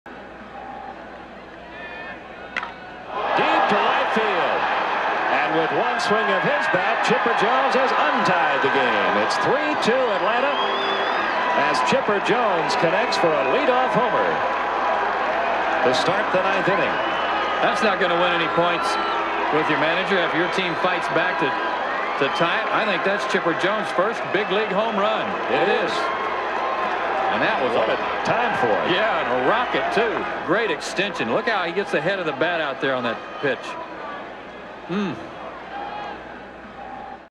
Chipper Jones connects for his 1st career Major League HR | May 9, 1995 @ Mets
5/9/95: Chipper Jones hits the 1st HR of his big league career on the road at Shea Stadium against the Mets